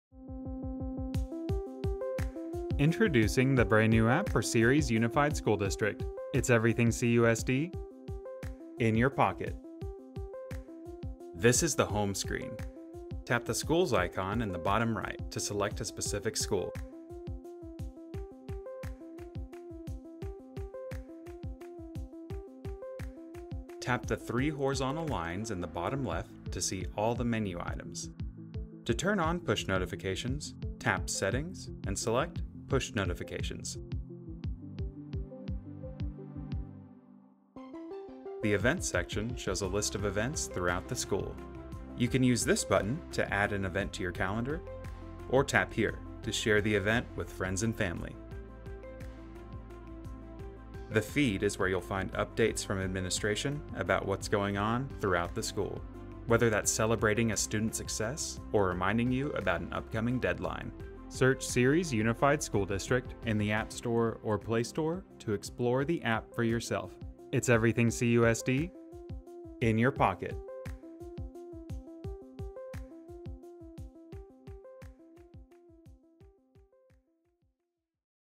CUSD Mobile App
Introducing the CUSD mobile app! Stay up to date with CUSD news and information from any device, wherever you are.